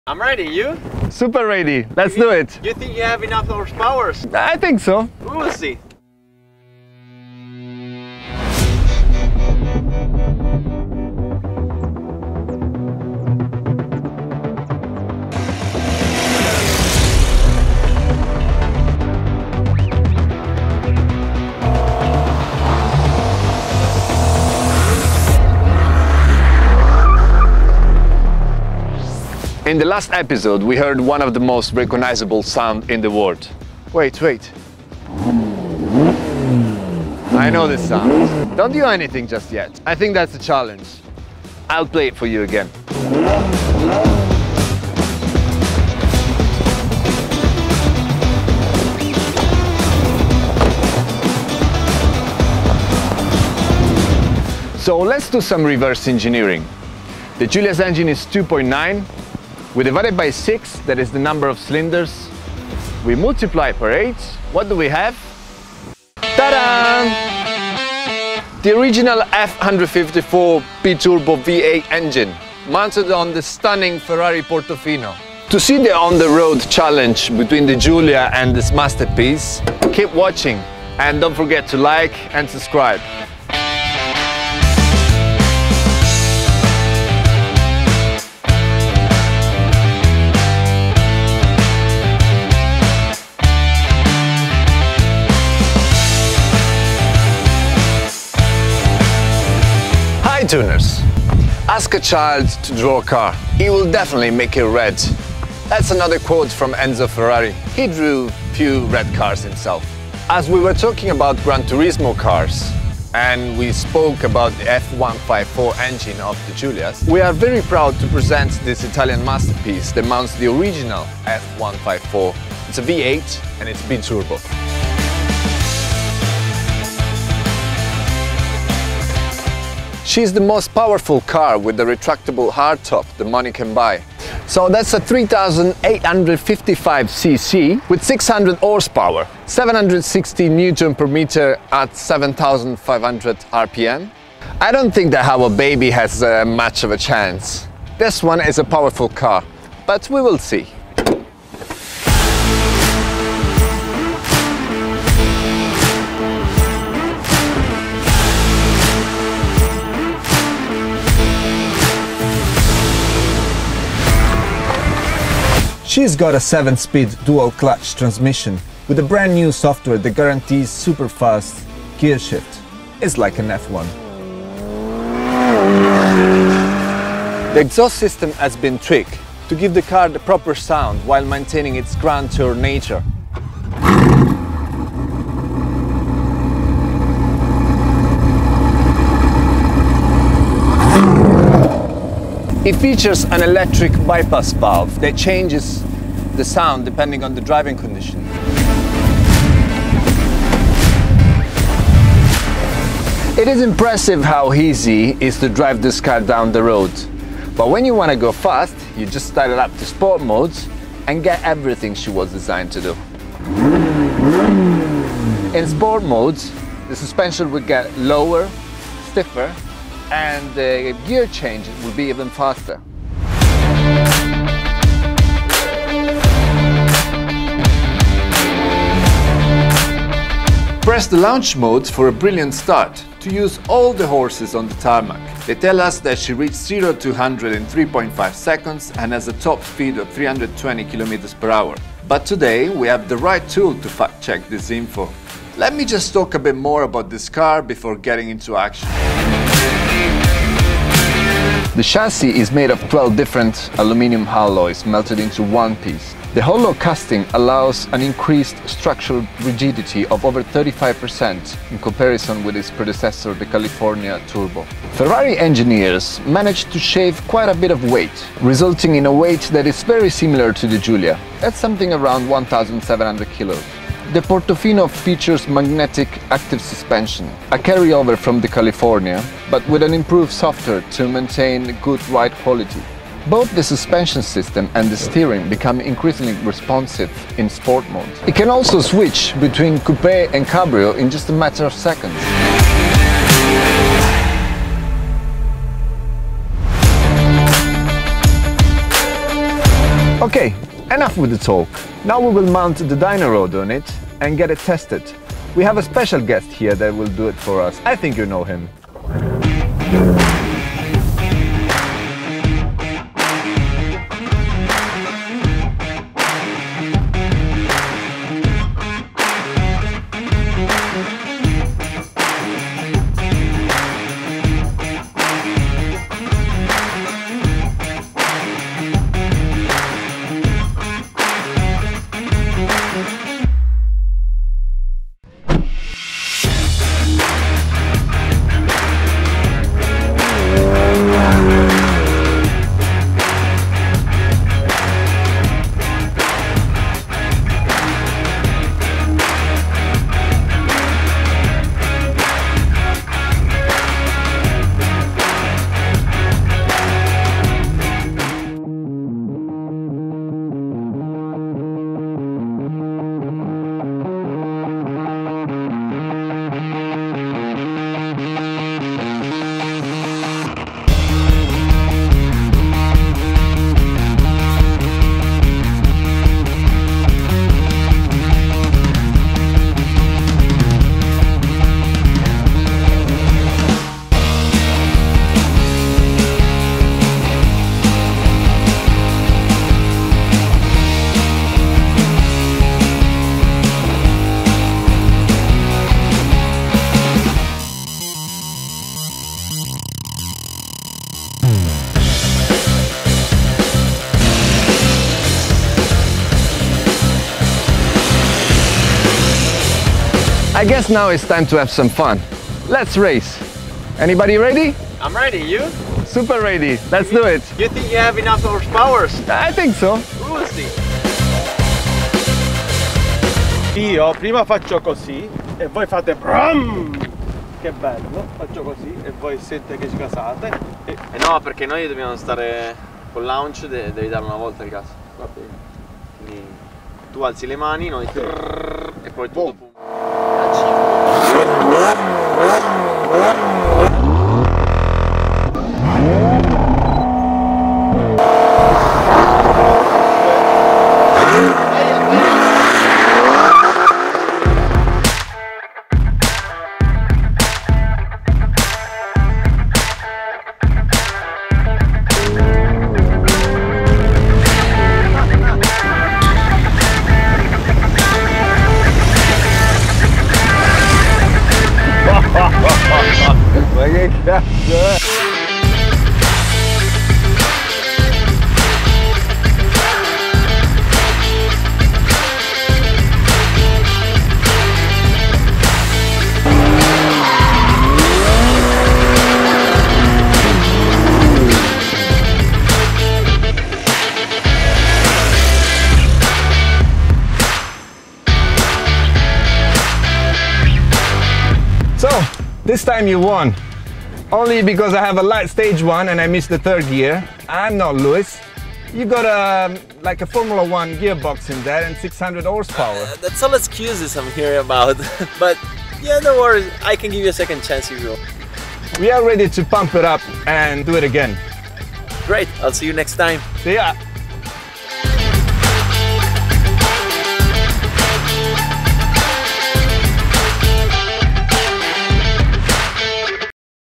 FERRARI Portofino vs Tuned ALFA ROMEO Giulia Quadrifoglio: the CHALLENGE, with NASKA 54
In this episode of Born To Tune, Daniel found a good challenge for himself and the Remapped Giulia Quadrifoglio,

A 600 hp Ferrari Portofino, the most powerful coupè with rectractable hardtop that money can buy!

We wanted to know if the 600 hp declared by Ferrari are real so we tested with our portable dynoroad and off course raced up with our Giulia !!!

Which will be the fastest?